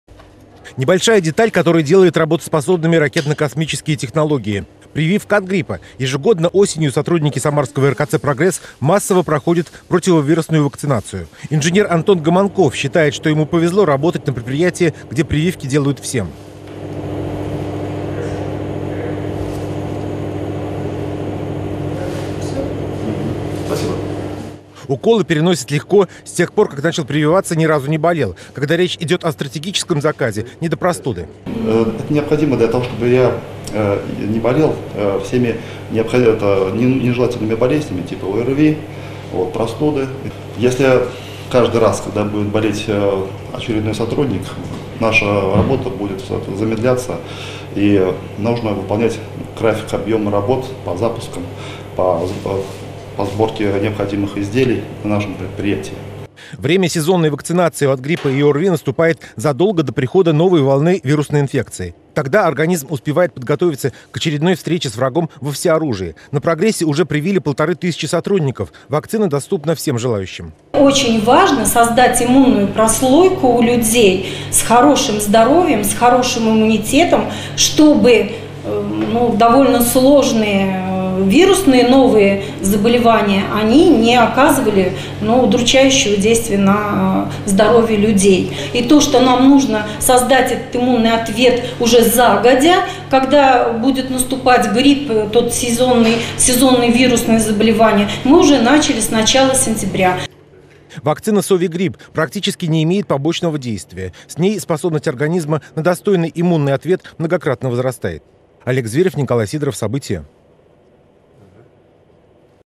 На РКЦ "Прогресс" всем сотрудникам делают прививки от гриппа
Время сезонной вакцинации против гриппа и ОРВИ. Врачи настоятельно рекомендуют всем сделать прививку перед тем, как придет волна простудных заболеваний. В самарском РКЦ "Прогресс" прививочная кампания идет полным ходом. Это — важное условие космических программ, — убедился Олег Зверев.